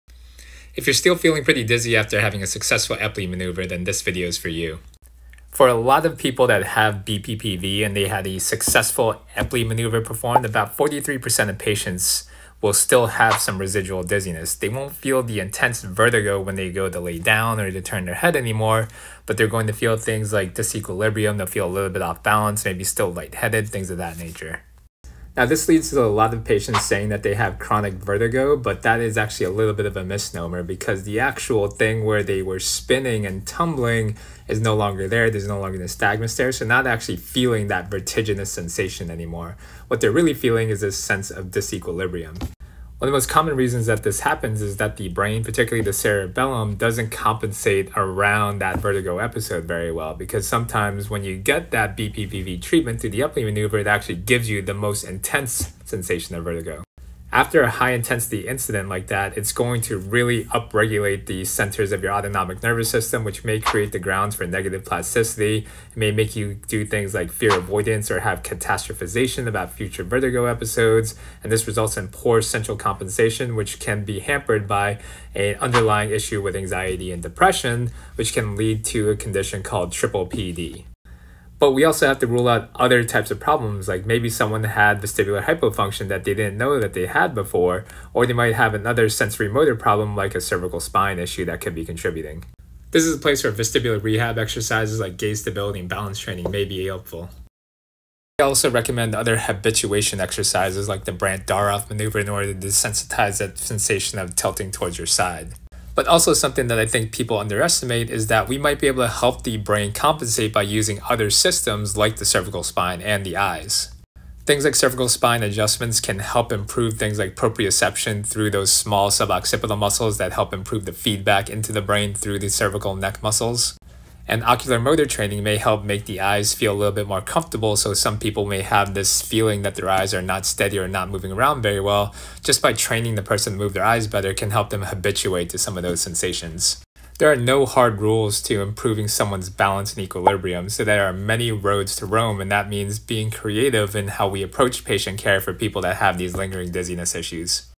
Still dizzy after getting the Epley maneuver?
Dr Chung addresses common concerns after treating with the Epley maneuver for BPPV